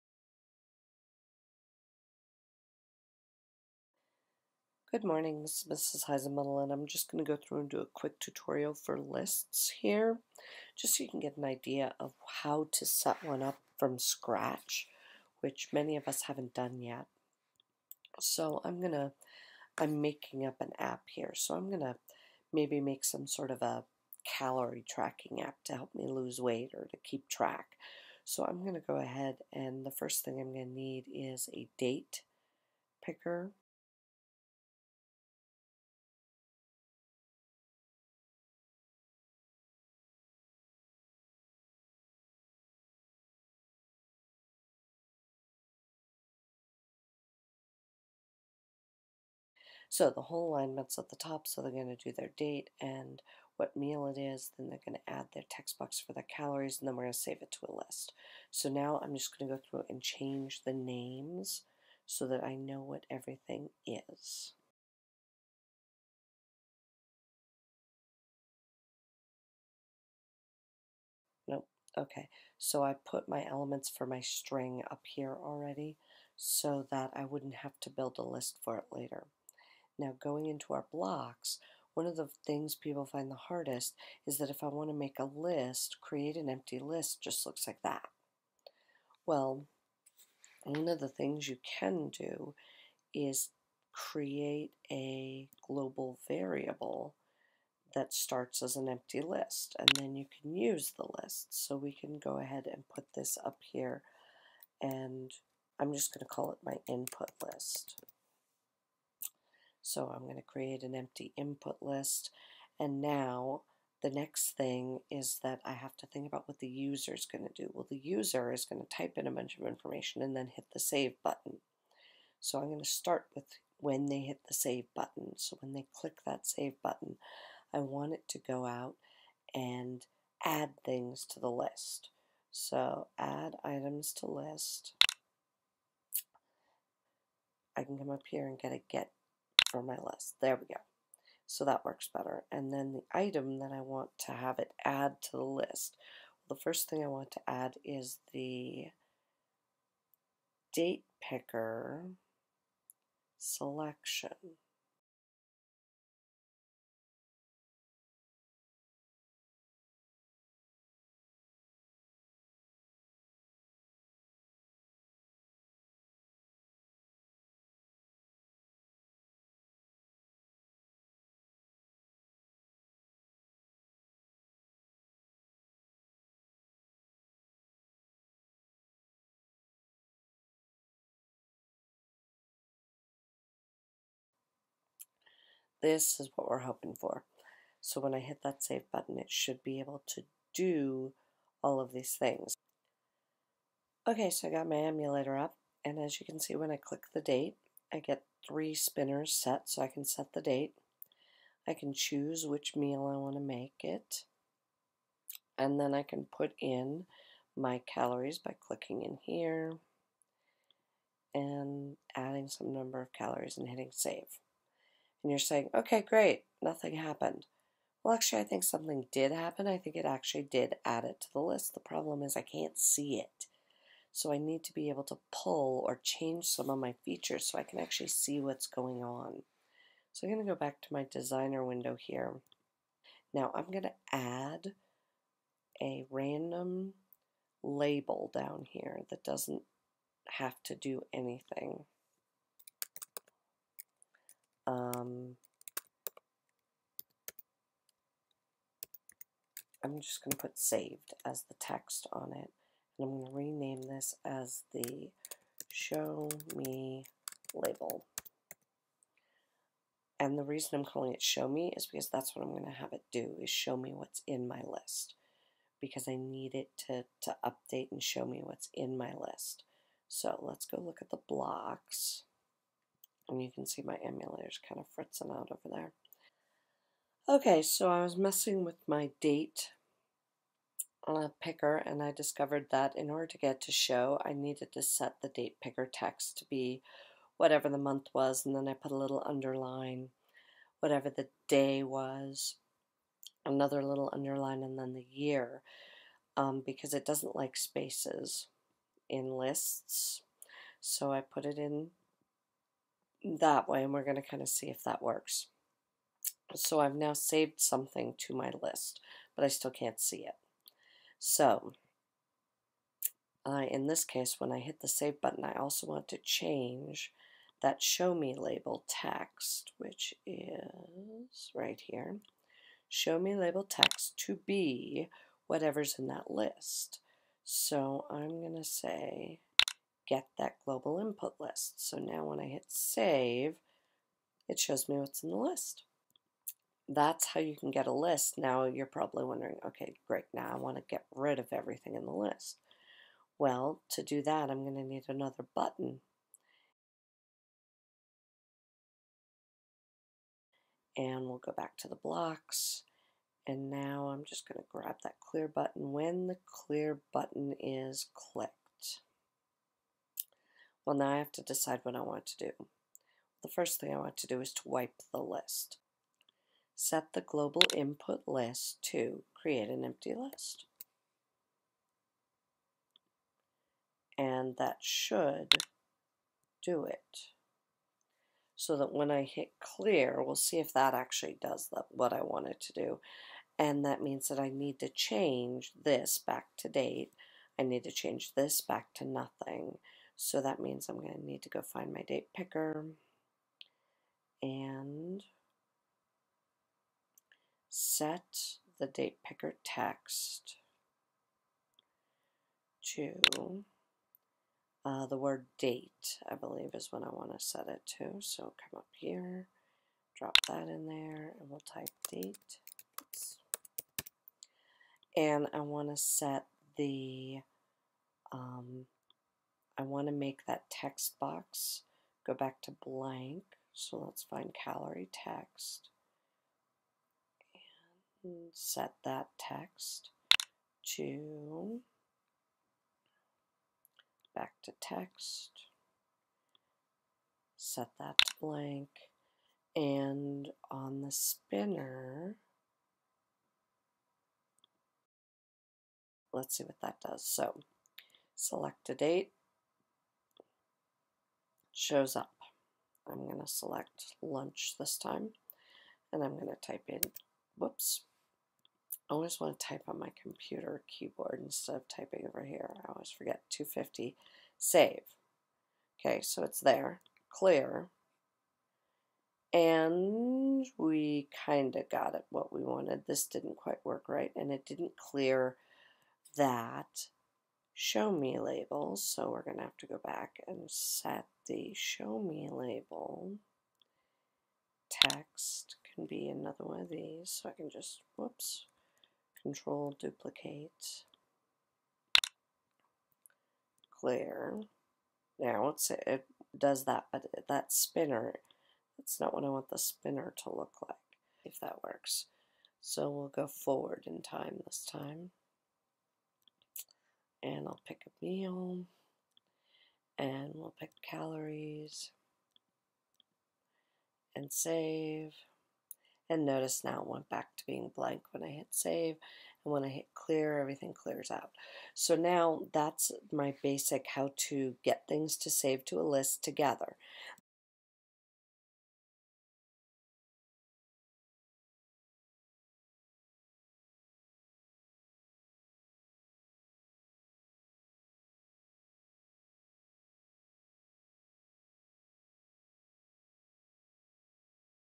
Lists and Spinners in MIT App Inventor
This is a quick tutorial about how to start the using lists and spinners from scratch. For the .aia file I created go and it should make you a copy of the file.